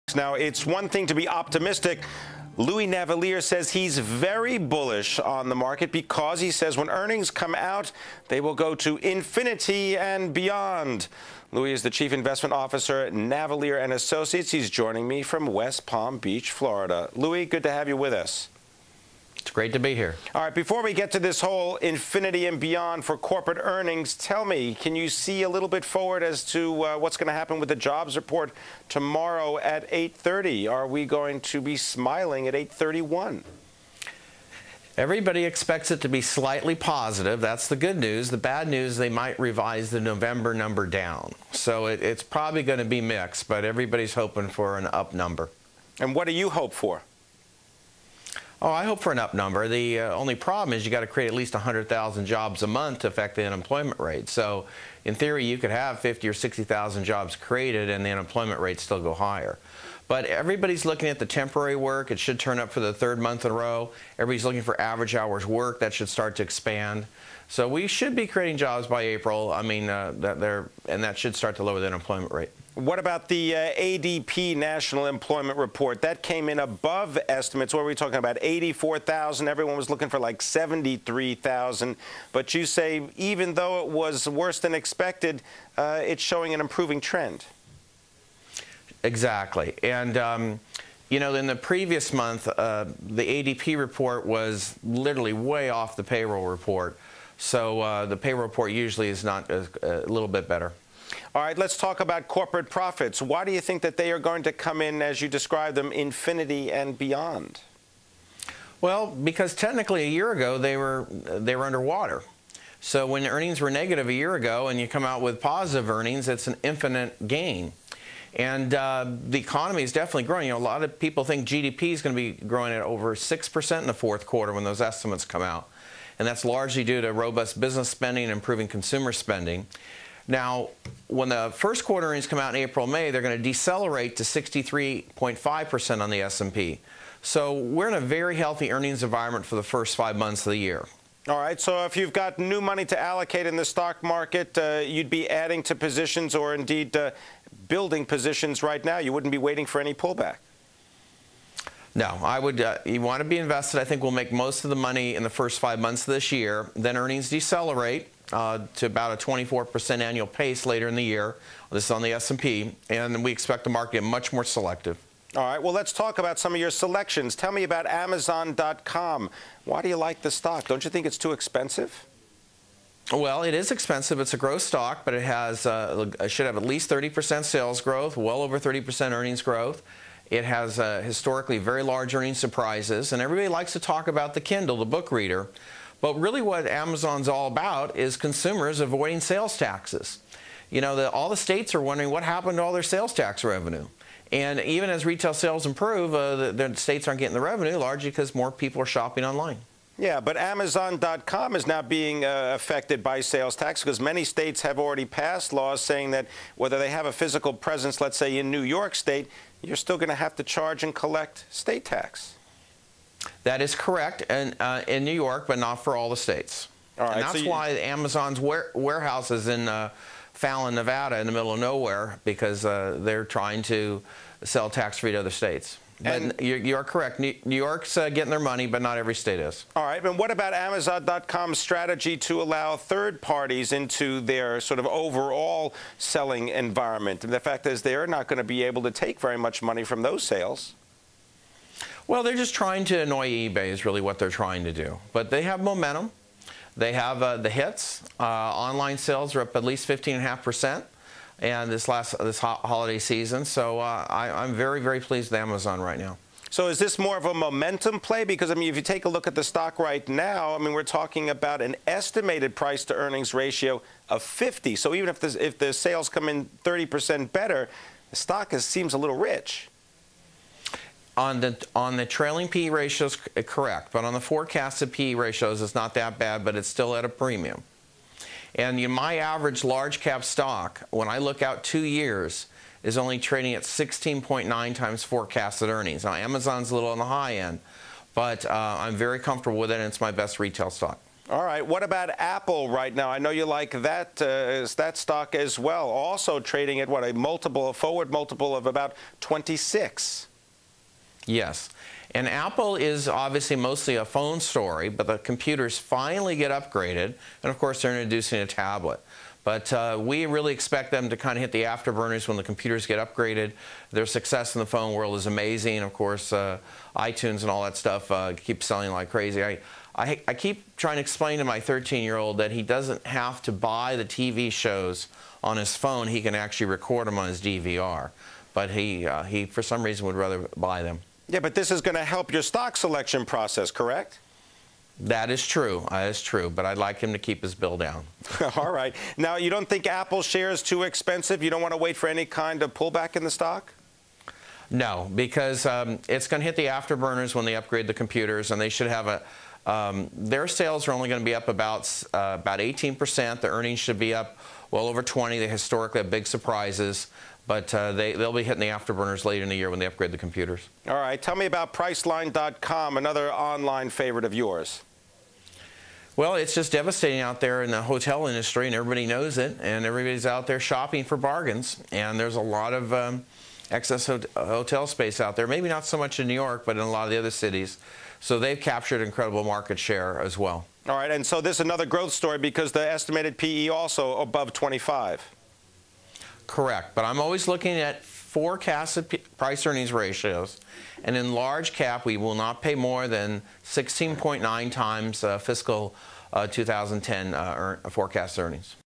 Louis Navellier Recommends Amazon, Apple, Priceline: Video
Jan. 7 (Bloomberg) -- Louis Navellier, chief investment officer of Navellier & Associates, talks with Bloomberg's Pimm Fox about his investment strategy.
     Navellier also discusses the outlook for December's U.S. employment data. (Source: Bloomberg)